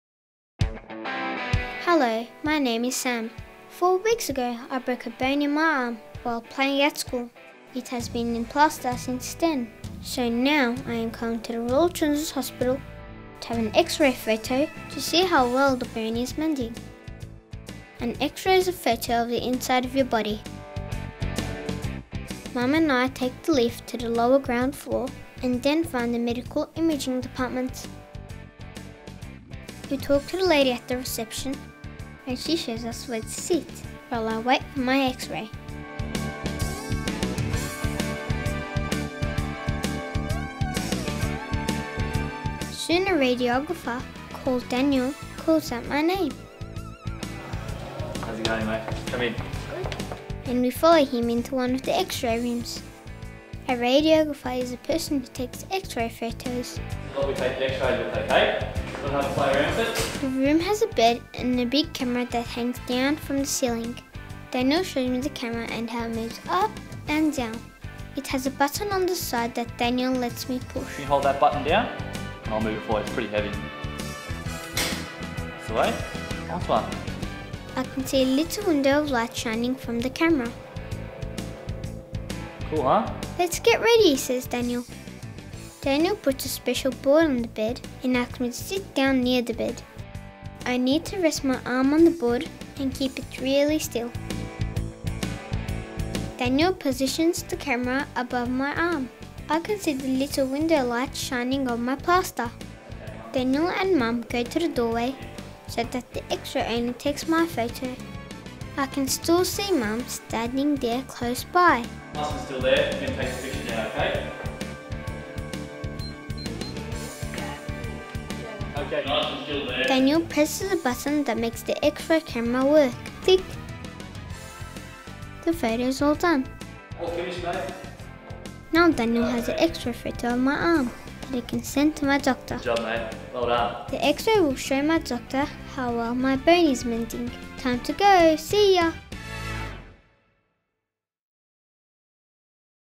A child's guide to hospital: Bone X-Ray
Sam gets an X-Ray picture taken of his arm so the doctor can see how the bone is mending. He meets Daniel the Radiographer at the RCH.

If you have any feedback or questions about this video please send an email to: be [dot] positive [at] rch [dot] org [dot] au